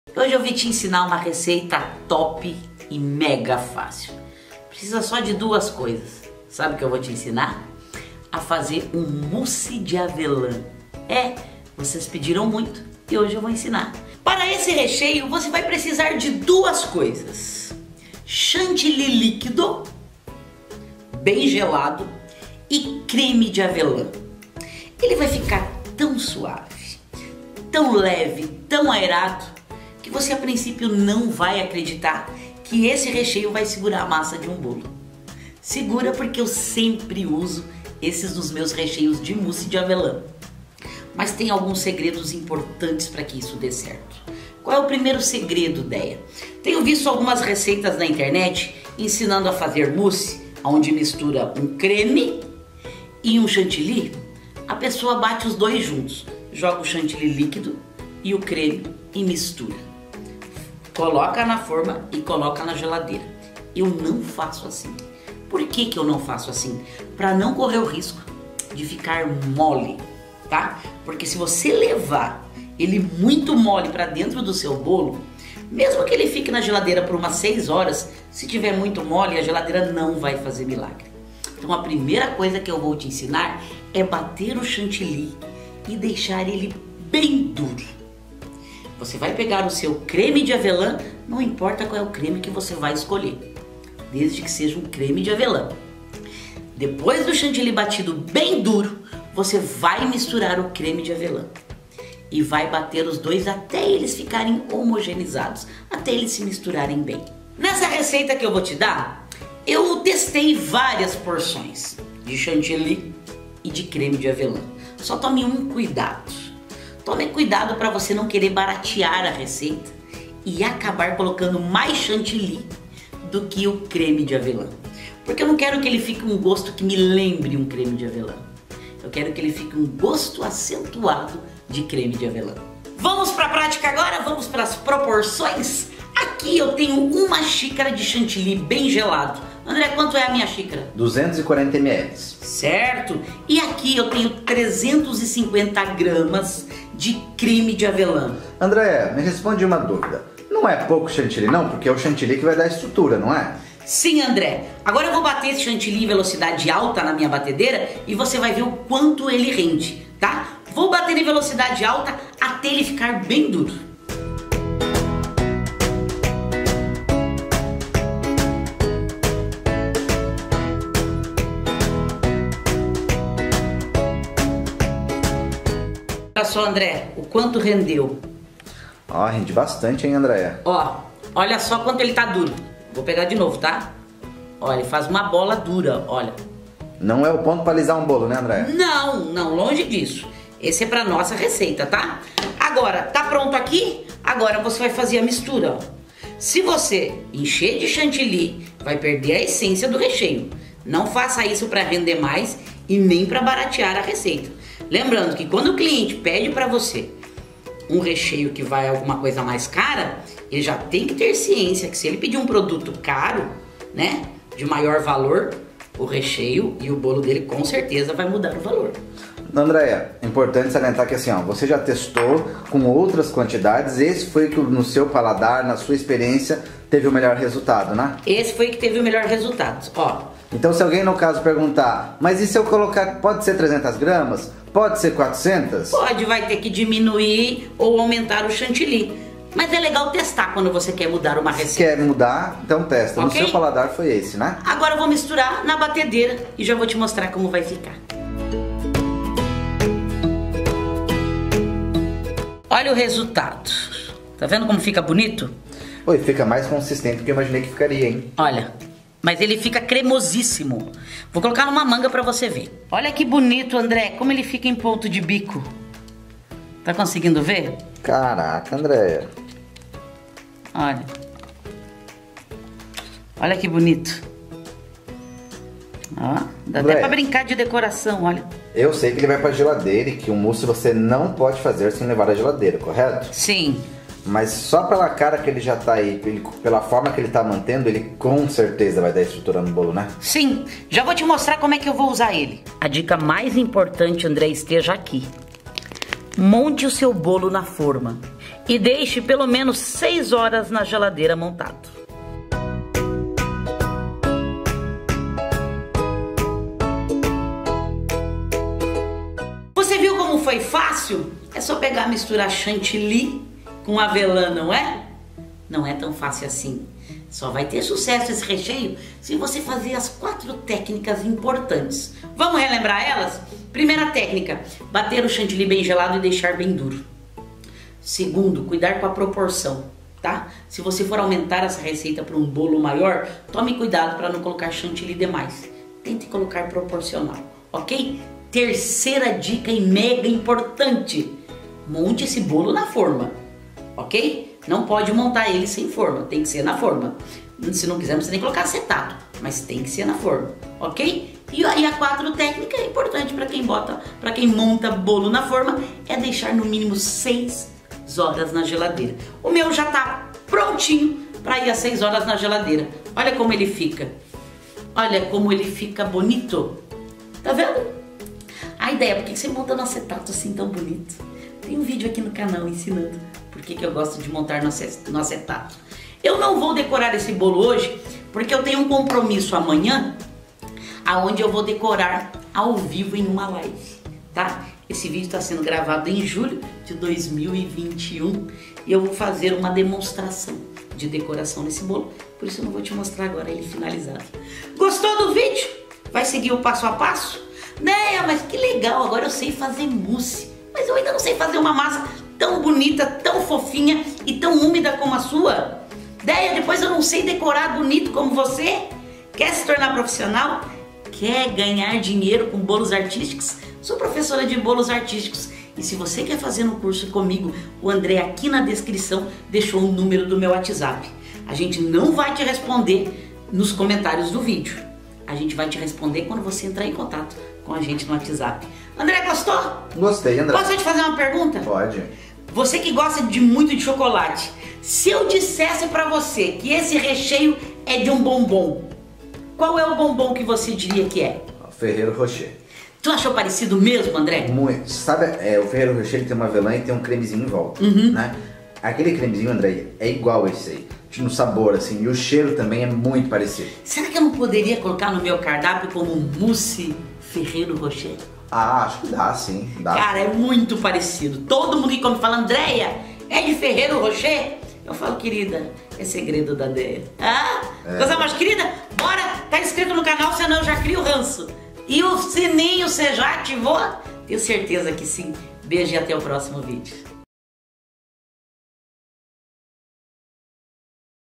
RECHEIO PARA BOLO COM APENAS 2 INGREDIENTES - NÃO VAI AO FOGO
Ingredientes:
240ml de chantilly líquido (bem gelado)
350g de creme de avelã (Nutella ou similar)

Whatsapp para informações sobre cursos: (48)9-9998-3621

🎥Direção: André Rodrigues
     Edição: André Rodrigues
                  Joelma Rodrigues